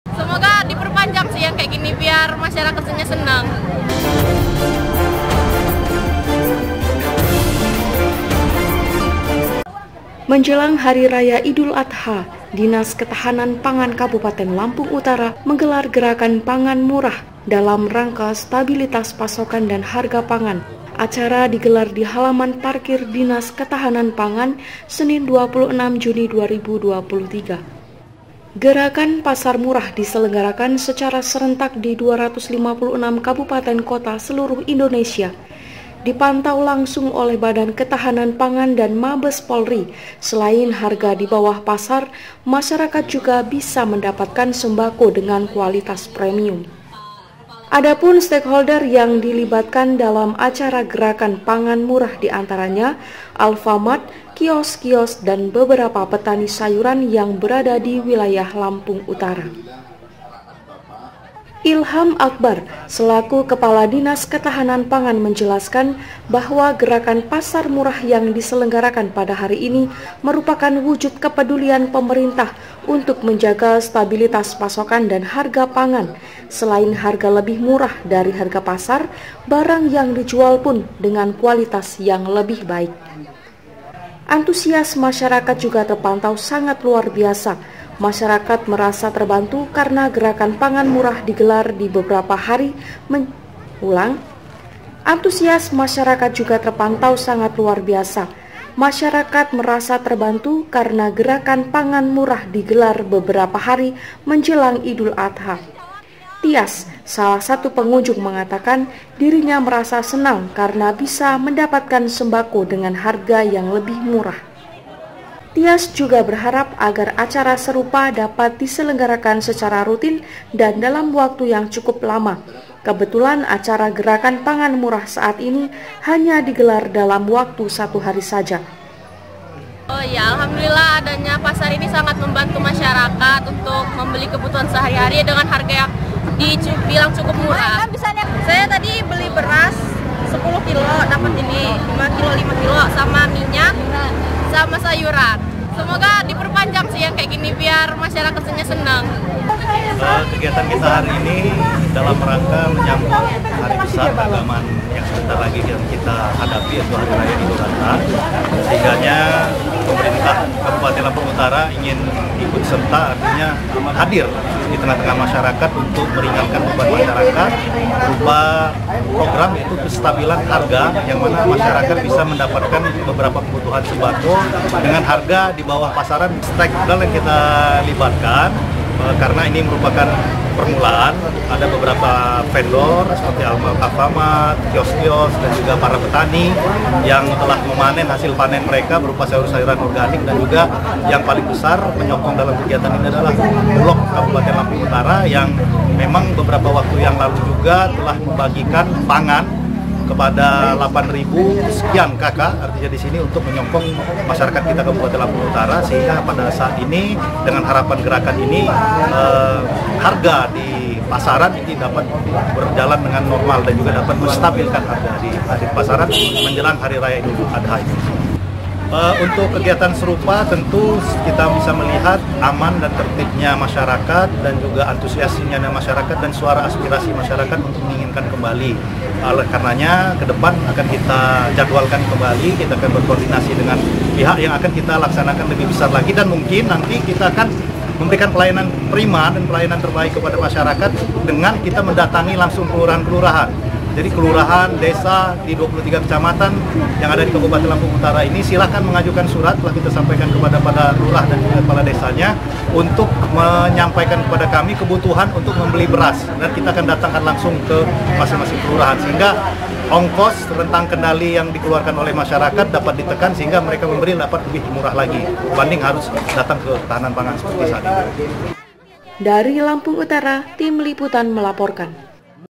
DINAS KETAHANAN PANGAN SELENGGARAKAN GERAKAN PANGAN MURAH
DINAS KETAHAN PANGEAN SELENGGARAKAN GERAKAN PANGAN MURAH, SENIN 26 JUNI 2023

INDEPENDEN / AKTUAL / INSPIRATIF